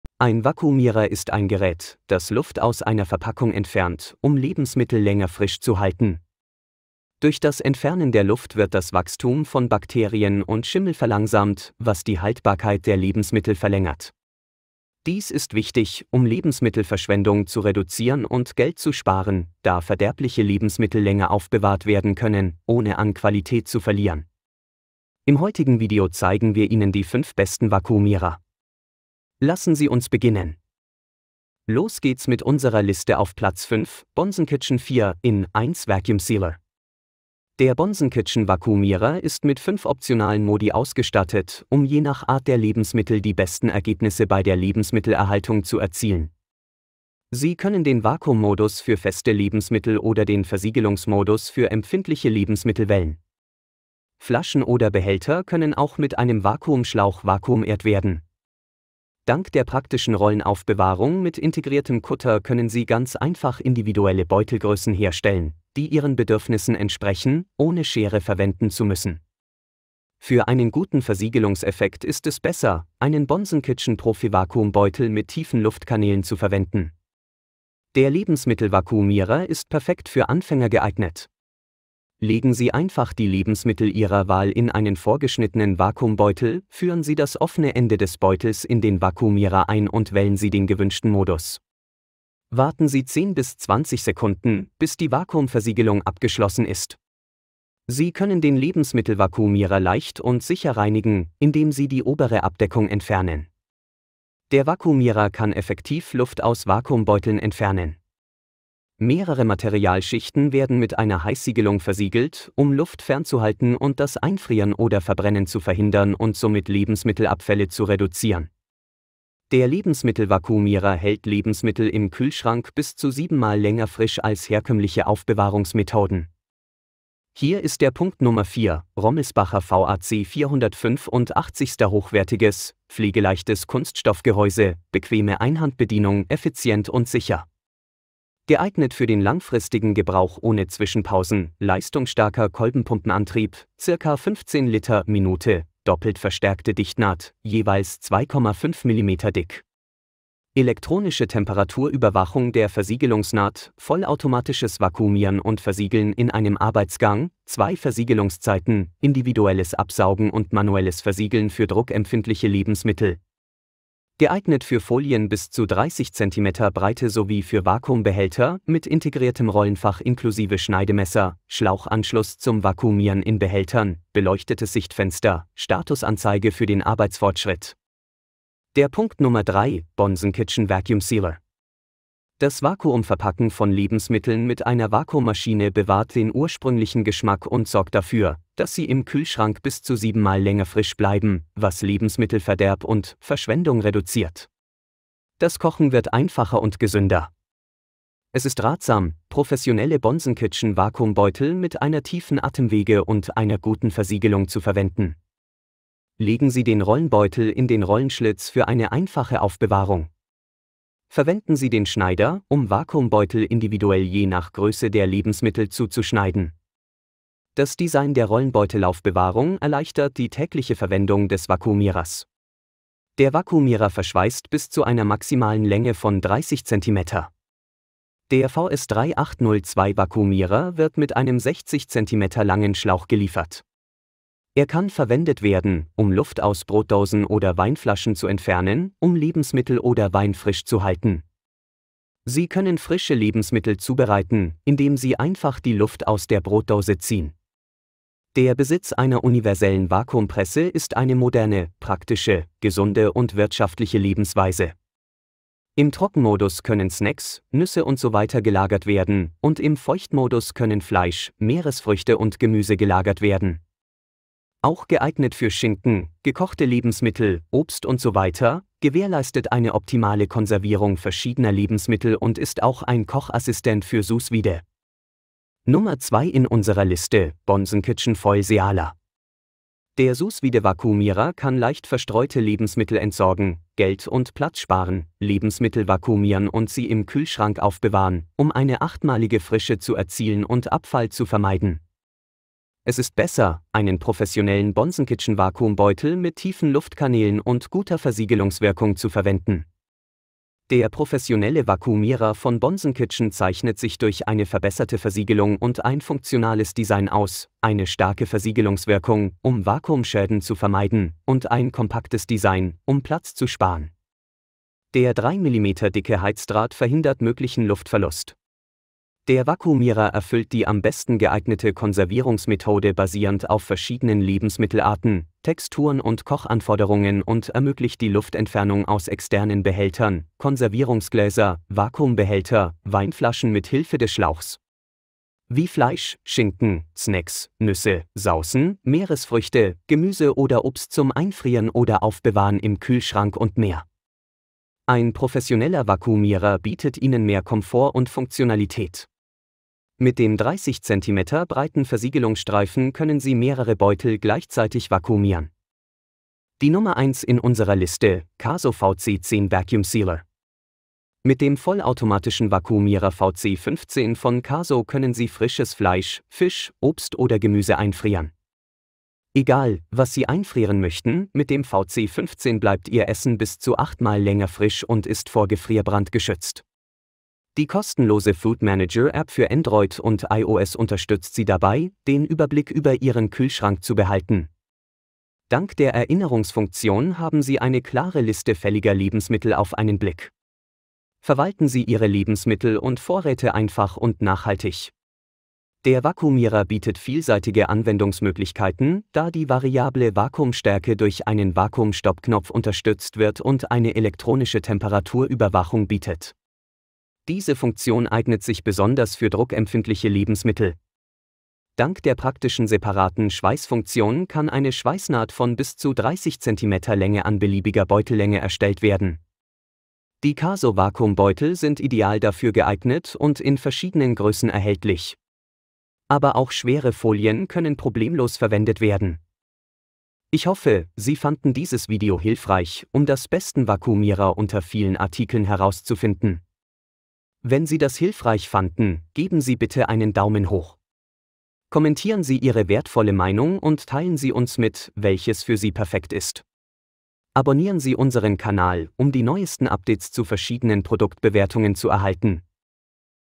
Besten Vakuumierer im Vergleich | Top 5 Vakuumierer Test
5. Bonsenkitchen 4-in-1 Vacuum Sealer

4. Rommelsbacher VAC 485

3. Bonsenkitchen Vacuum Sealer

2. Bonsenkitchen Foil Sealer

1. CASO VC10 vacuum sealer